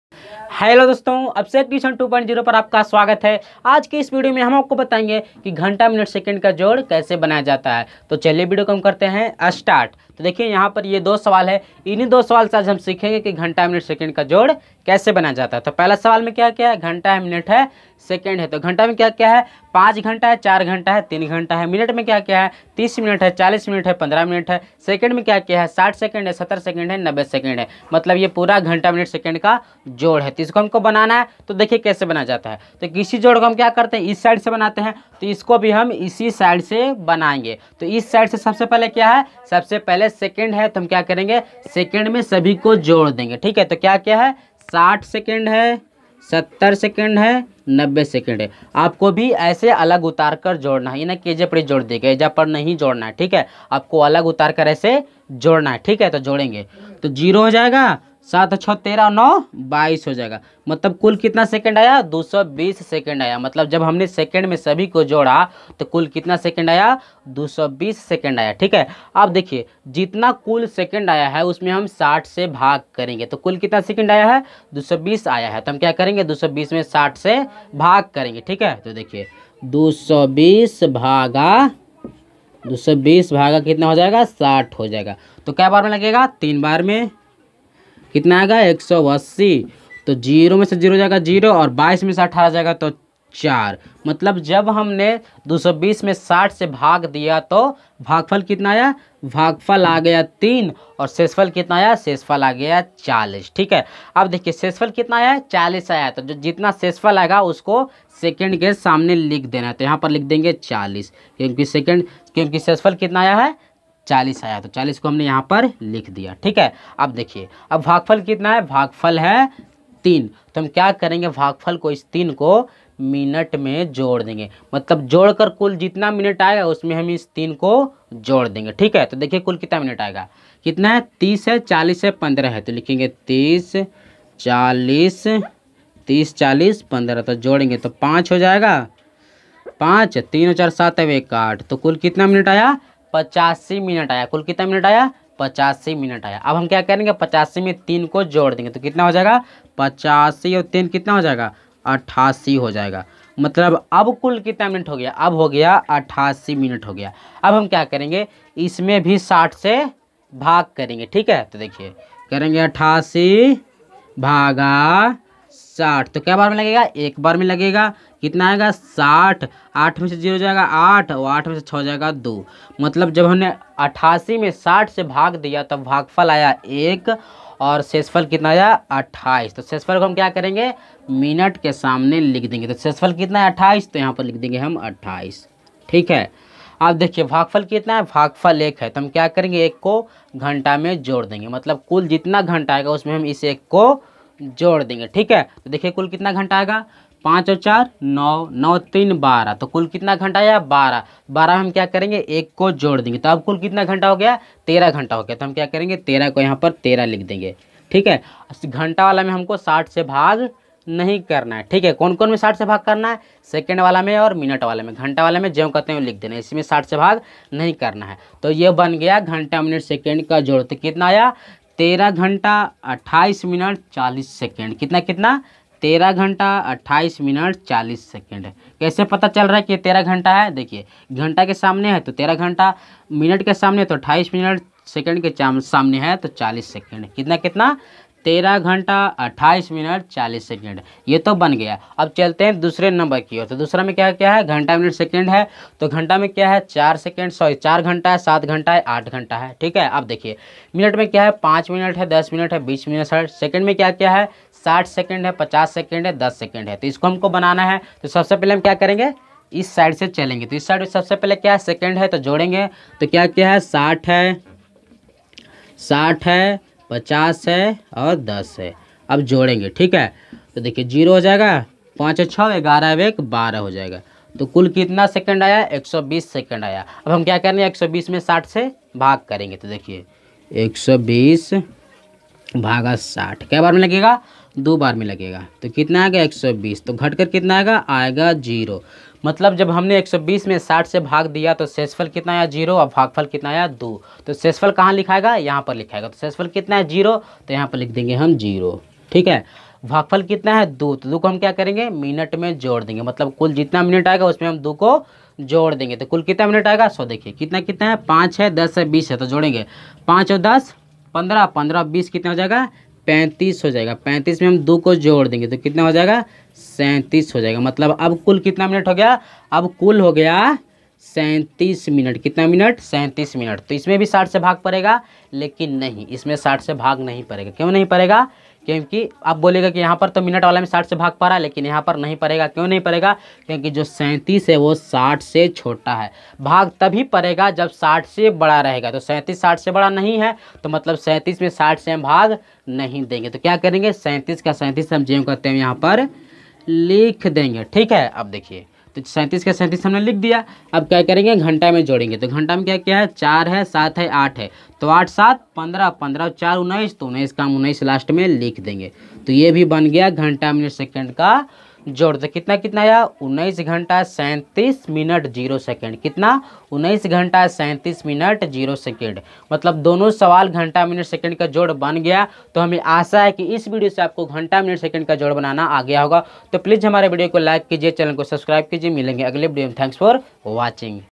घंटा मिनट सेकंड के जोड़ को बनाना सीखे|ghanta minute second ke jod ko banaye asani se
Hallo  dosto abhishek  tution2.0  par aapka  swagat hai  hamare channel  par aapko 8th,9th,10th and  basic math  ka videos  milta rahega